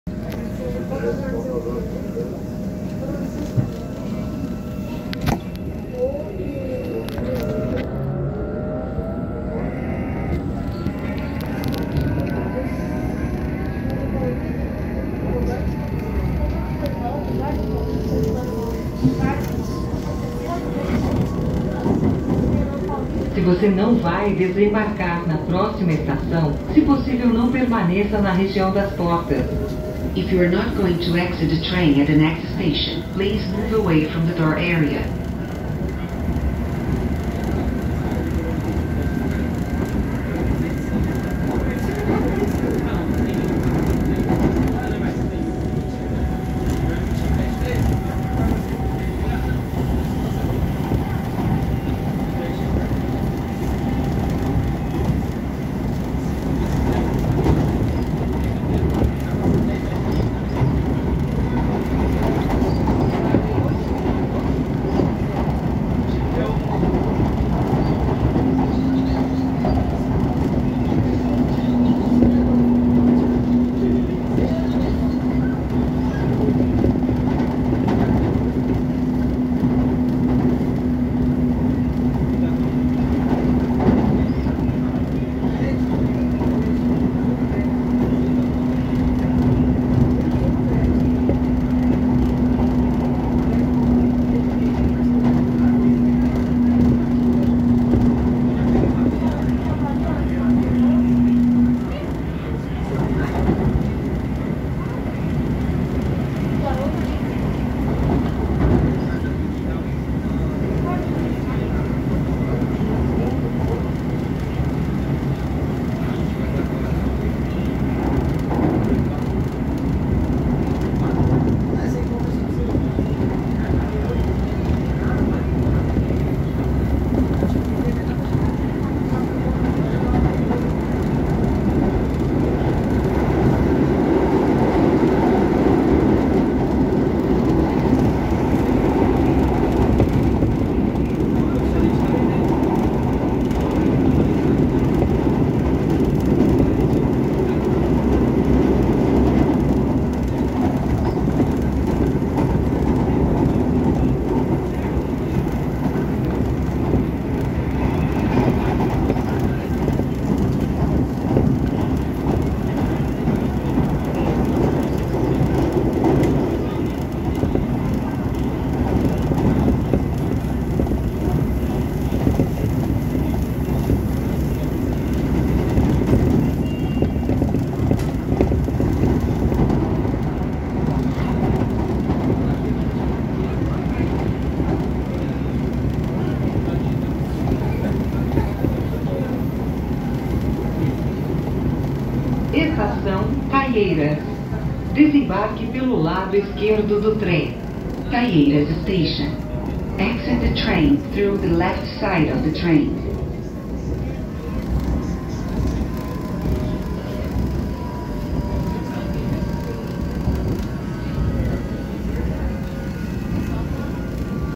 CPTM Trecho Série 9500 Entre As Estaçãos Perus Até Caierias Linha 7 Rubi (Serviço 710)
Olliver Fernando Bus Train